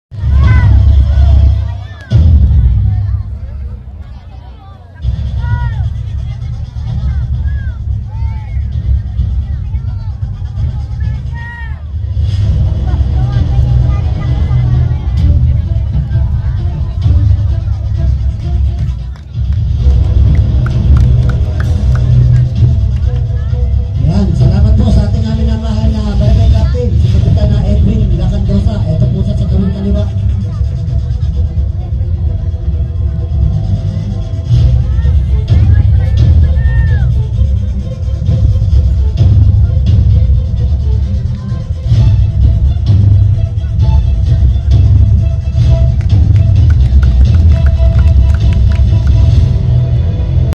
Bagting Fiesta 2016 presents Gutierrez Family Acrobat (Part 2/5) (FULL HD 1080p)
In the Philippines we call this "Zarsuela".
It's about showmanship of talents and skill with laughter.